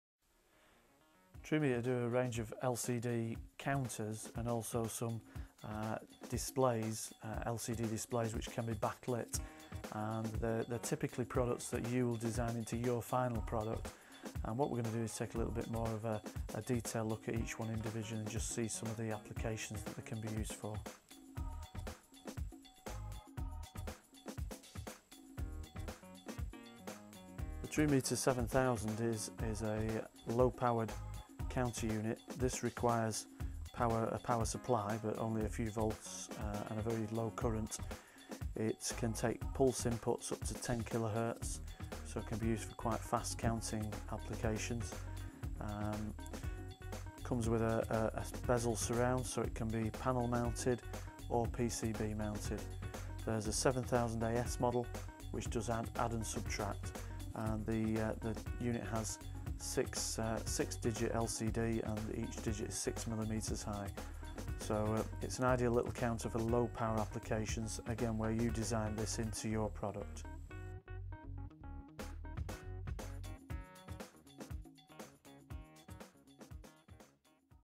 Trumeter 7000 LCD Counter Digital Display
Trumeter 7000 LCD counter by Countersales. This can be added to a circuit board or PCB. These are also used as a counter to regulate servicing and maintenance schedules.